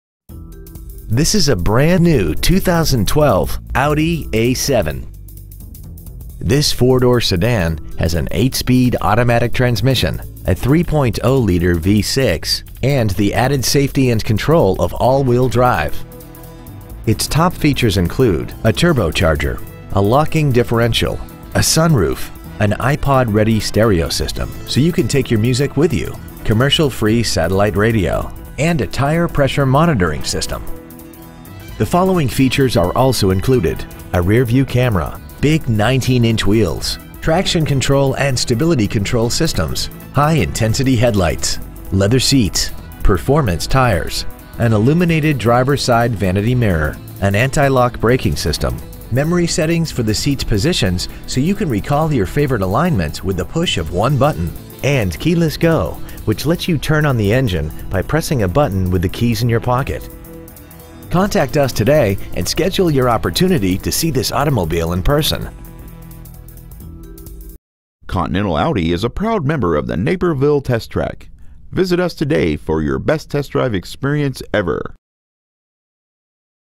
2012 Audi A7 Naperville IL 60540
Year : 2012

 Make : Audi

 Model : A7

 Engine : Supercharged Gas V6 3.0L/183

 Trans . : Automatic

 Exterior : Quartz

 Miles : 12

 Interior : Black

 Stock : C446

 1527 Aurora Avenue

 2012 Audi A7 Naperville IL